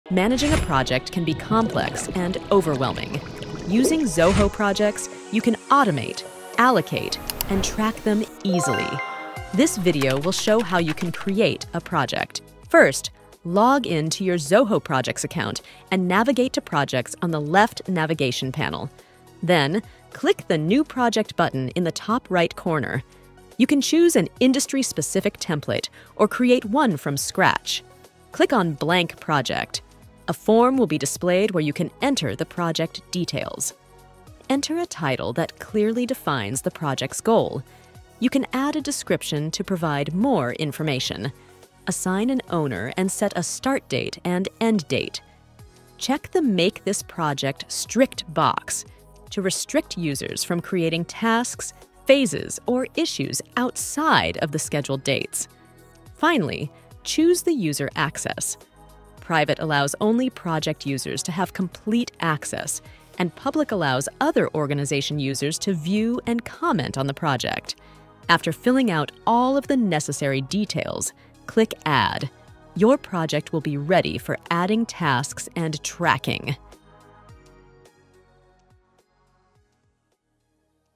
How to Create Project!
Learn how to quickly create projects from scratch using Zoho Projects, saving time and effort as a project manager. Whether you're managing Software, Manufacturing, Construction, Marketing, or Product Management projects, this video walks you through simple steps to get started quickly and effectively.